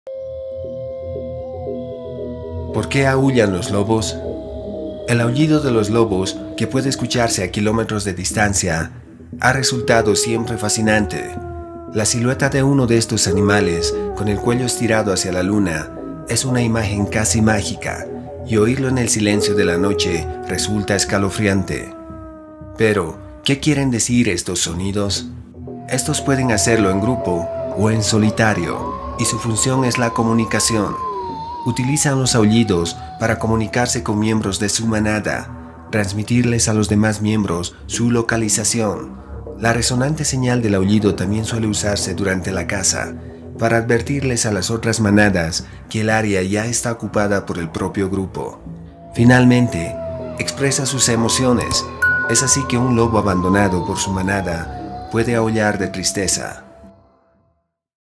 Por qué Aúllan los Lobos
Los lobos son animales especialmente conocidos por su sonoro e imponente aullido, sobre todo cuando se presencia la luna llena, pero ¿por qué aúllan?
Pues bien, los lobos tienen tendencia a aullar debido a varios motivos, para unir a su manada, para atraer a su pareja, marcar su territorio, para que sus enemigos se alejen, dar una señal de alarma o comunicar su posición en caso de sentirse en peligro.